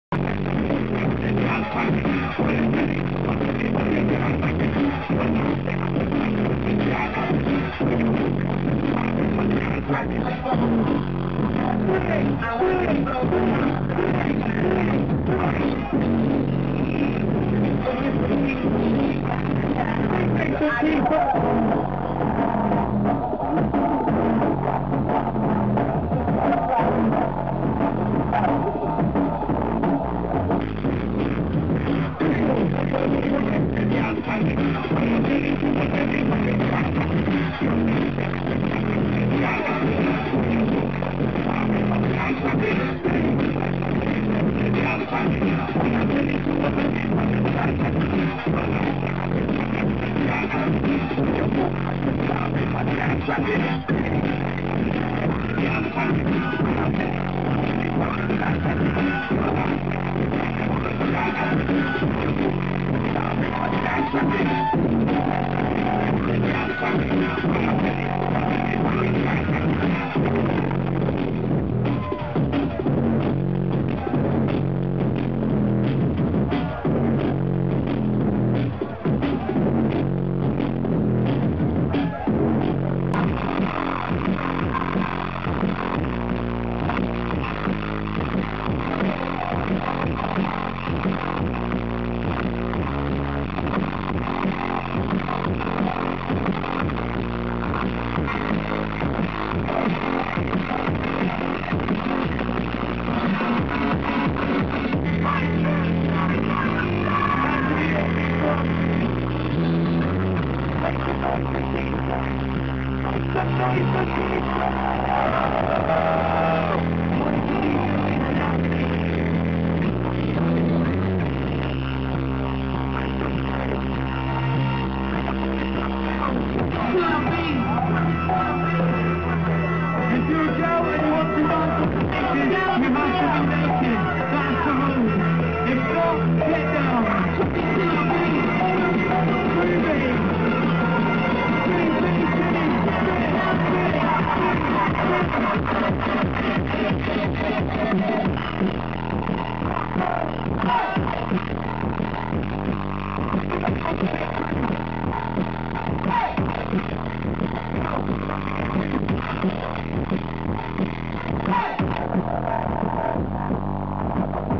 St Pauls Carnival 2010 - my favourite stage.
Sorry about the sound - it was REALLY loud out there and my camera just couldn't record it properly.